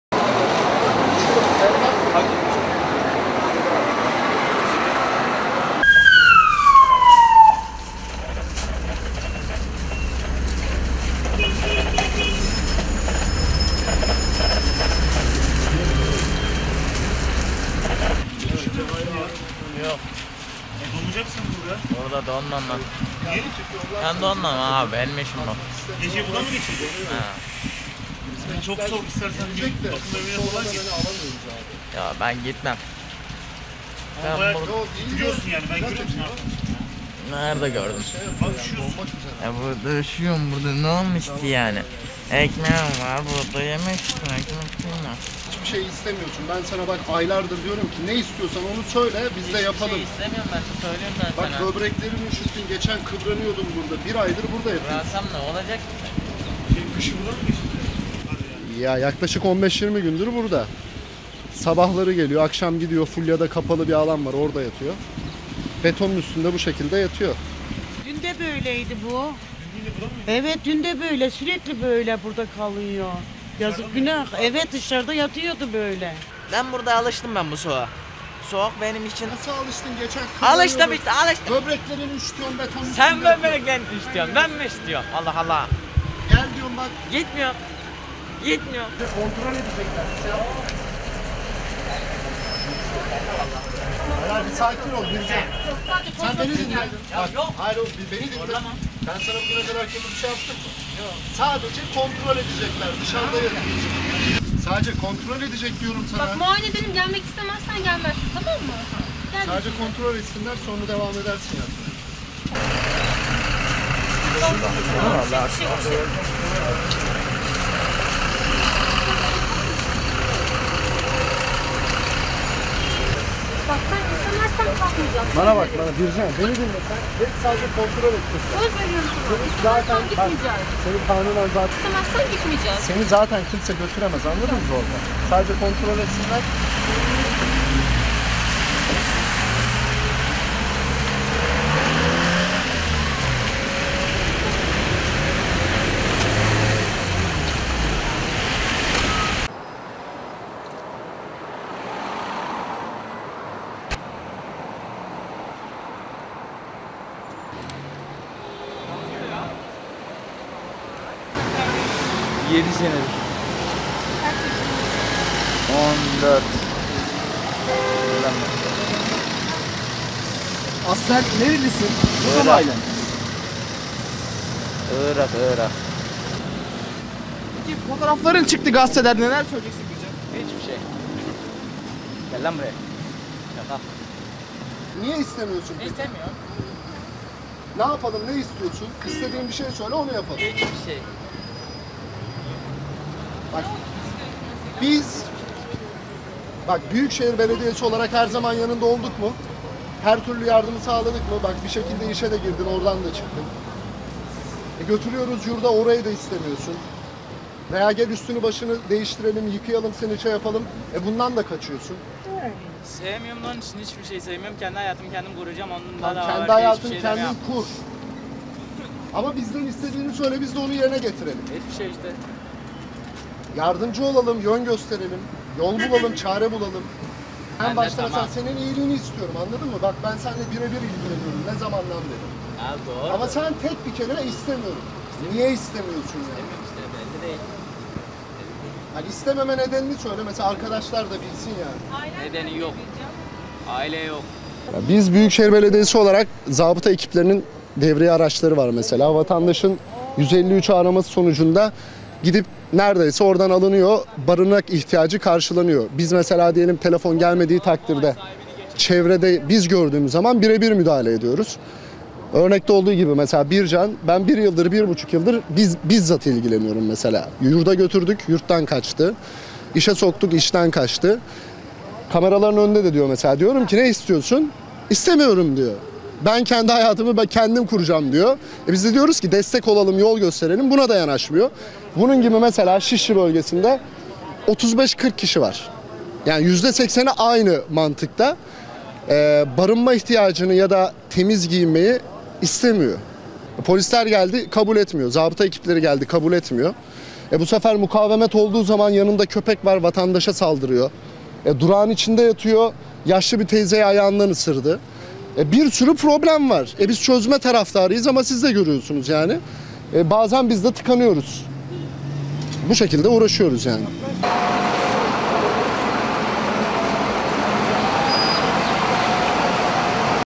Soğuk havada sokakta kalmakta inat etti!
Şişli'de soğuk havaya rağmen üzerine geçirdiği çuvallarla bir otobüs durağında yerde yatan genç, kendisini kapalı bir mekana götürmek isteyen belediye görevlilerine zor anlar yaşattı. Iraklı olduğu öğrenilen genç, uzun uğraşların ardından sağlık ekipleri tarafından kontrol için hastaneye götürüldü.Şişli Mecidiyeköy'de bir otobüs durağında soğuk havaya rağmen üzerine geçirdiği çuvallarla yerde yatan bir genç, belediye ekiplerini harekete geçirdi. Daha önce de aynı yerde köpeğine sarılarak uyuduğu fotoğraf sosyal medyaya düşünce gündeme gelen genç kendisini kapalı bir mekana götürmek isteyen belediye ekiplerine zor anlar yaşattı. Iraklı olduğu öğrenilen genç, belediye ekiplerinin ısrarlarına rağmen bulunduğu yerden ayrılmak istemedi. Dondurucu soğukta yerde yatan genç, belediye ekipleri tarafından ikna edilemeyince olay yerine sağlık ekipleri çağrıldı.
İstanbul Büyükşehir Belediyesi Destek Hizmetler Daire Başkanlığı görevlisi, durakta kalan genci bir türlü ikna edemediklerini belirterek, "Yaklaşık 15-20 gündür burada. Sabahları geliyor, akşamları Fulya'da kapalı bir alan var orada yatıyor. Betonun üstünde bu şekilde yatıyor. Dün gece burada değildi. Sürekli takip ediyoruz" diye konuştu.
"SOĞUĞA ALIŞTIM"
Üşümediğini belirten genç, hiçbir yere gitmek istemediğini ifade ederek, "Üşümüyorum, ben donmam. 3-4 gündür buradayım. Irak'tan 10 sene önce buraya geldim. Ben burada bu soğuğa alıştım" diye konuştu.
Gencin sürekli durakta kaldığını ifade eden bir kadın ise, "Dün de böyleydi, sürekli burada kalıyor. Yazık, günah" dedi.
Sürekli durakta kaldığı öğrenilen genç, olay yerine gelen sağlık görevlileri tarafından ikna edilerek ambulansa bildirildi. Gencin, kontrol edilmek üzere hastaneye götürüldüğü öğrenildi.